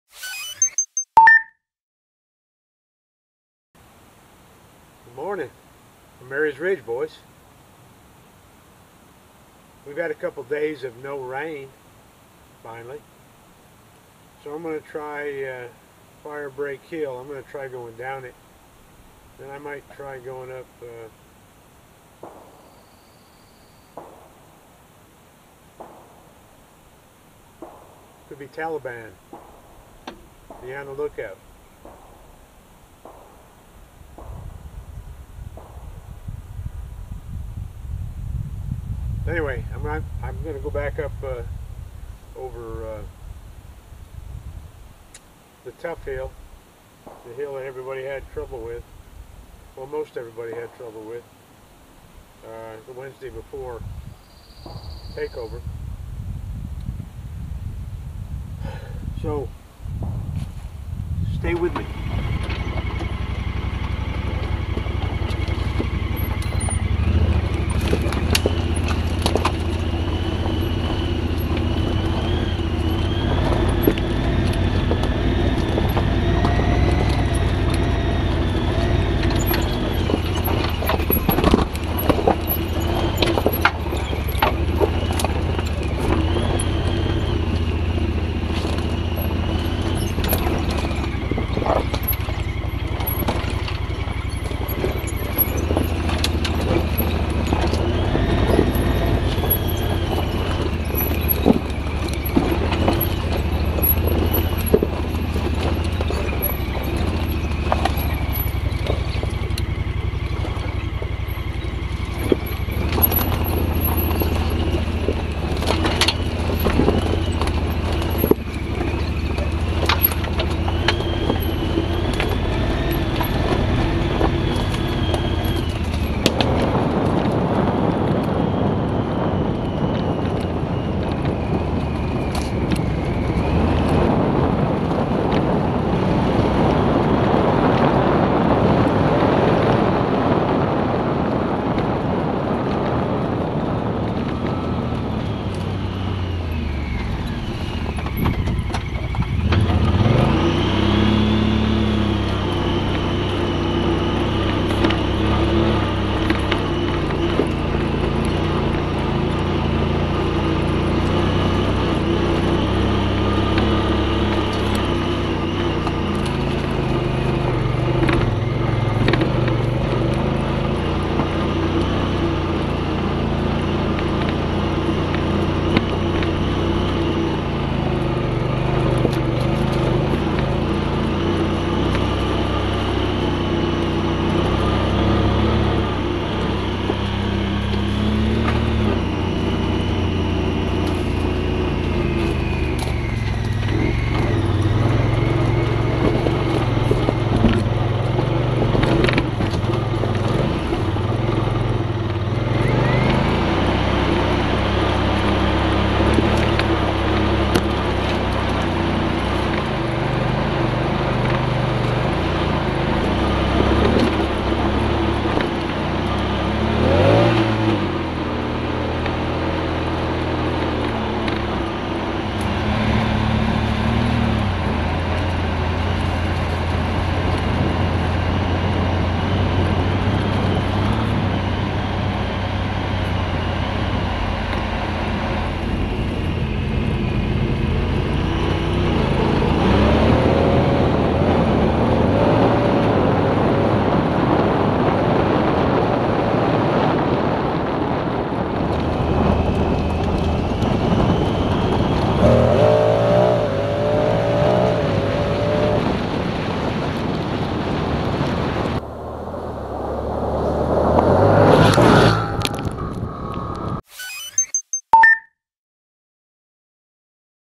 Mary's Ridge Morning Run
Finished up pressure washing the deck and the deck furniture so thought I'd go play a little. I'm blessed to have this area to play on.